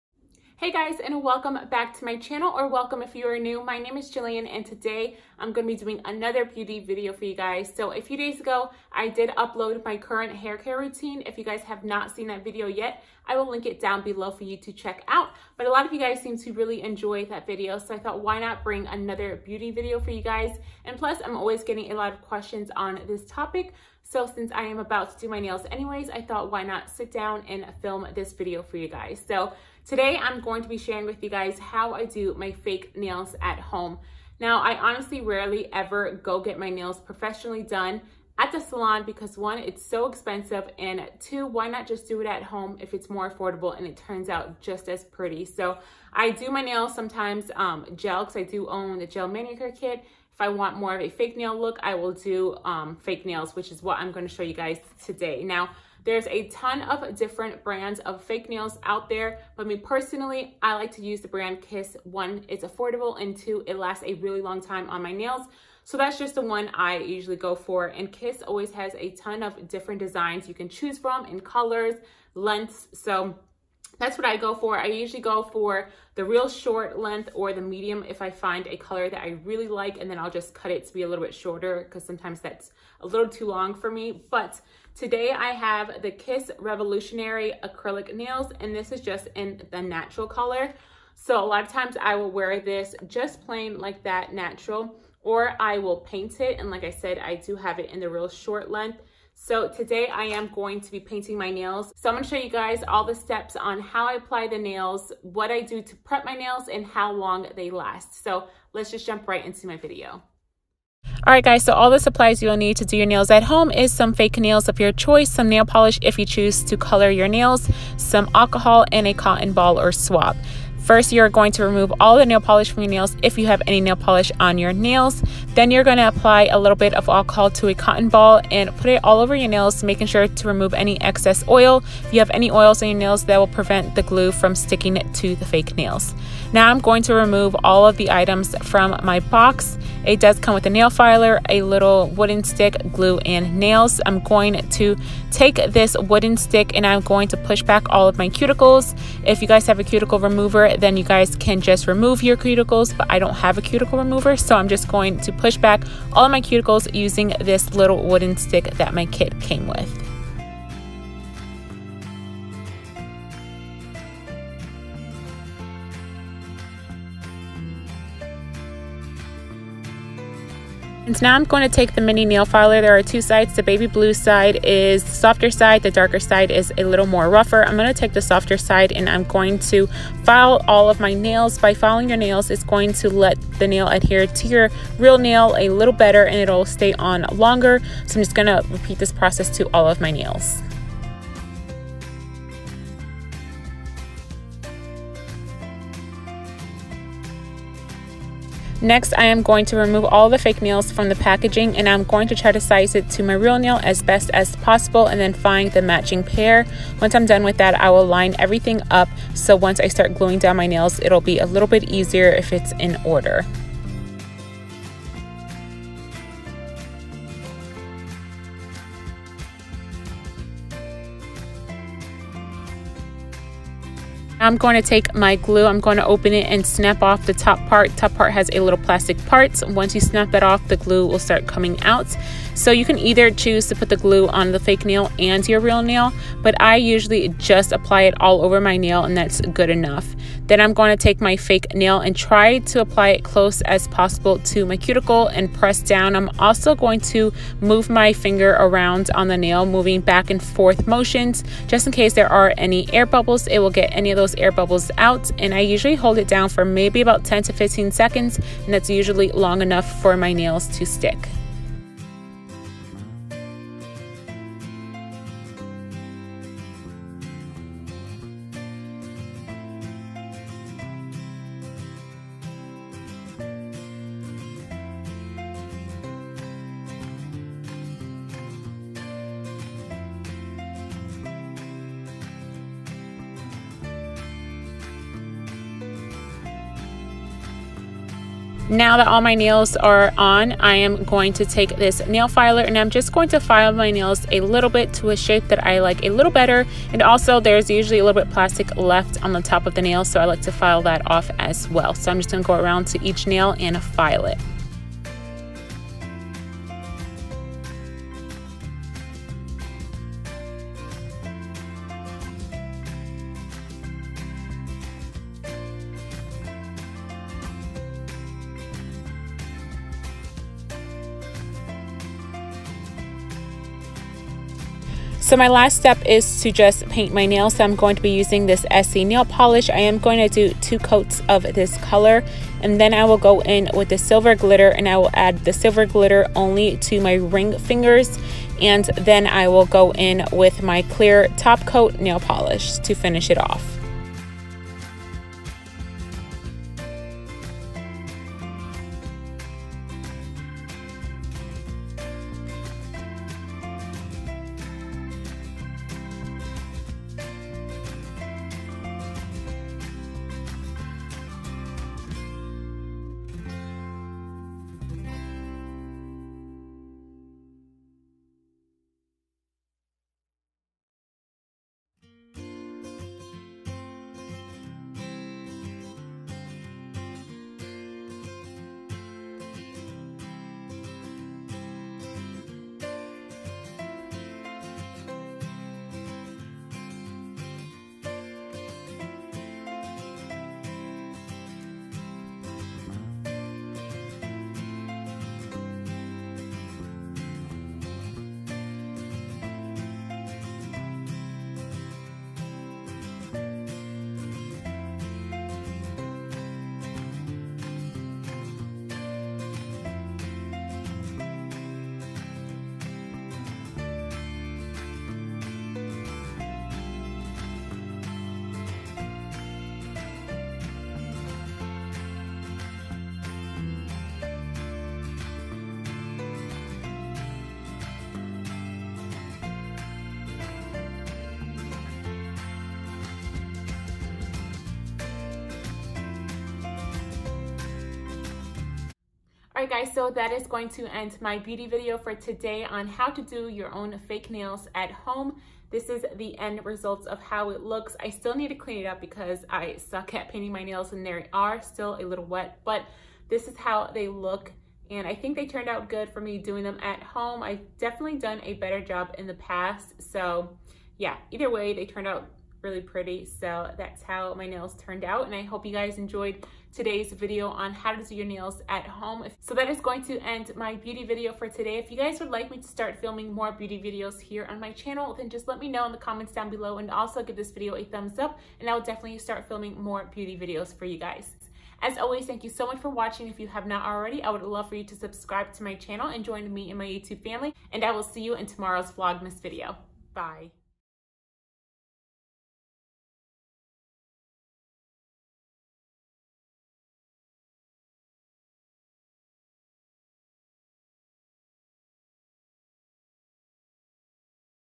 VLOGMAS DAY 15 | DIY Fake Nails At Home
Hey friends! Today I have another beauty video for you. I show you how I do my fake nails from home. I’ve gotten so many questions about it, so I figured I would film a video on how I do them. Hope you enjoy today’s vlogmas video.

Filming Equipment

iPhone X
Selfie Light
Tripod Stand

Music Credit